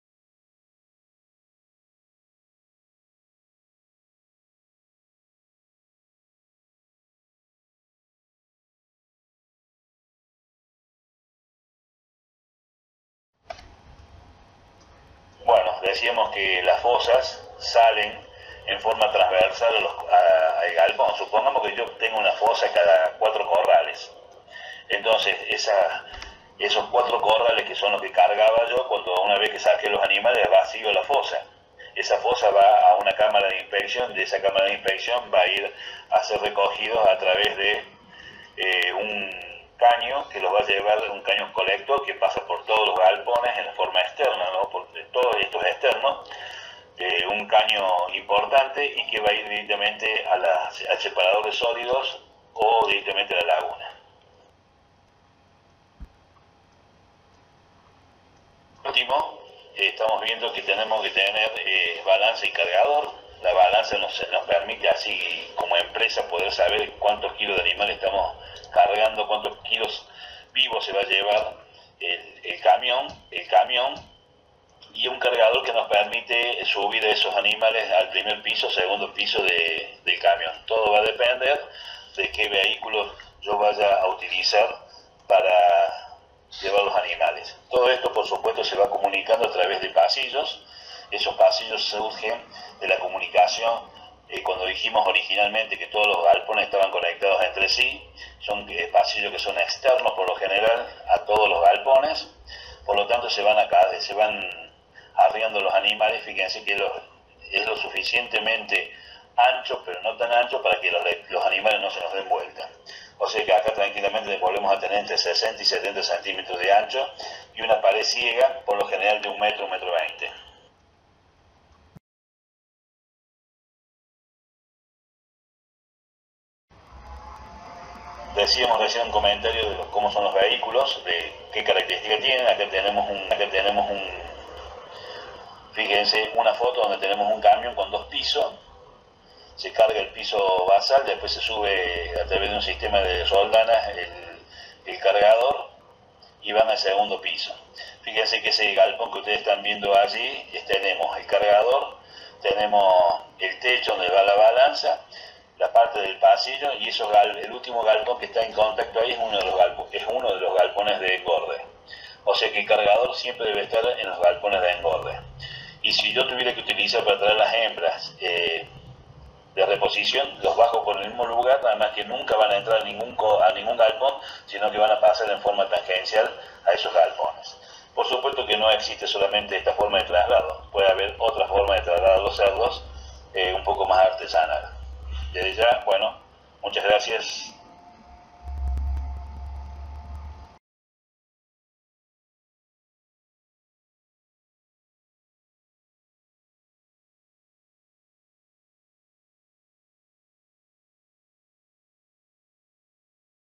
Instalaciones para Sistemas Confinados (Parte 7)
Espacio Curricular: Sistemas Pecuarios Alternativos: Producción Porcina
Tema: Instalaciones para Sistemas Confinados (Parte 7)
Docentes: Daniel Caramello
Facultad de Ciencias Agropecuarias de la Universidad Nacional de Córdoba